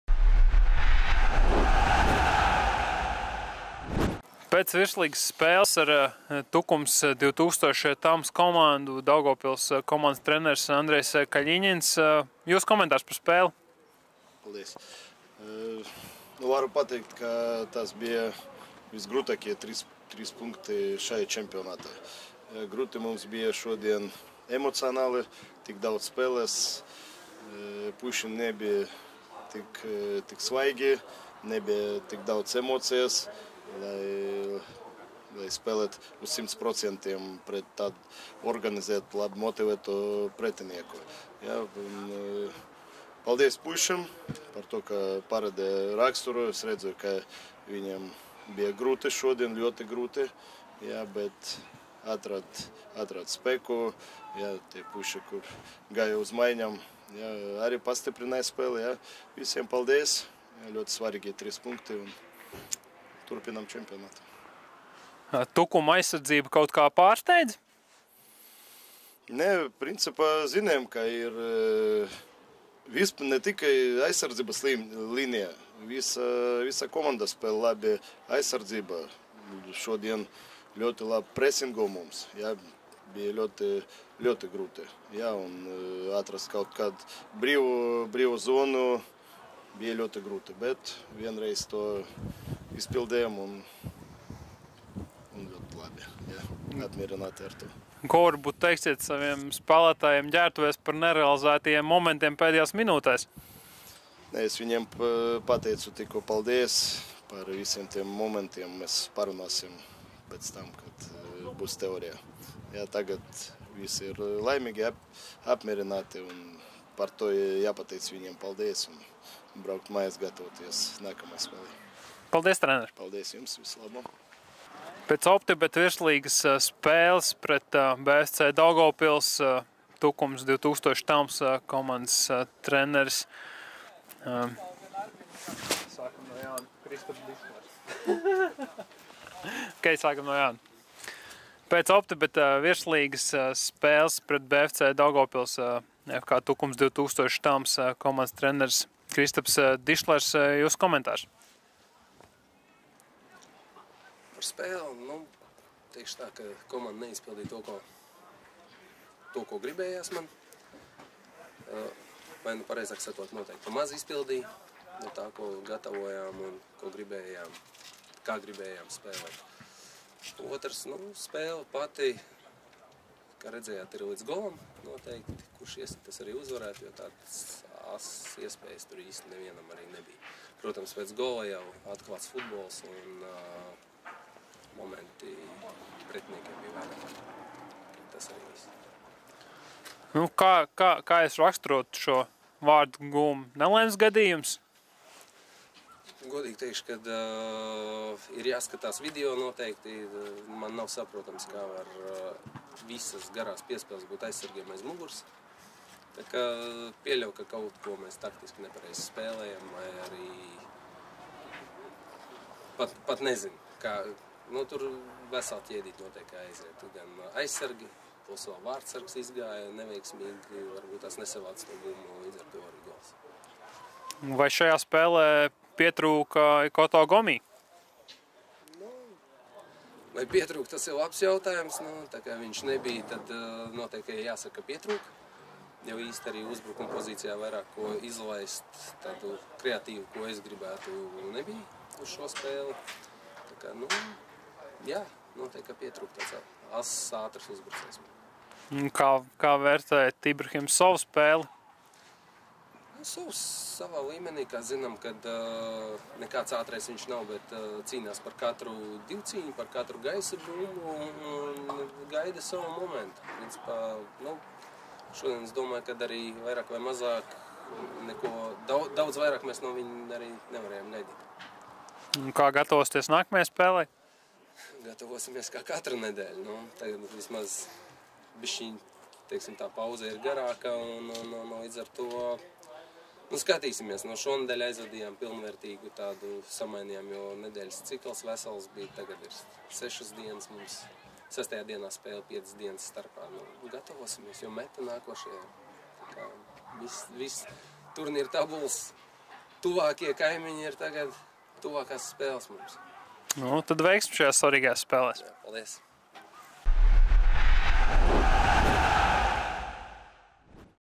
10. kārta: FK Tukums 2000/Telms 0:1 BFC Daugavpils (Preses konference)
💬 Andrejs Kaļiņins (BFC Daugavpils): Komanda neizpildīja to, ko man gribējās, jeb pareizāk sakot izpildīja maz. Spēle bija līdz vārtiem, uzvarēja tas, kas iesita. Pēc vārtiem protams bija atklāts futbols un tad pretiniekiem momentu bija vairāk.

💬 Kristaps Dišlers (FK Tukums 2000/Telms): Varu pateikt, ka šie bija visgrūtākie trīs punkti šajā čempionātā. Nebija viegli emocionāli, ļoti saspringts grafiks, puiši šodien nebija svaigi, lai mēs varētu spēlēt uz visiem 100% pret tik labi organizētu pretinieku.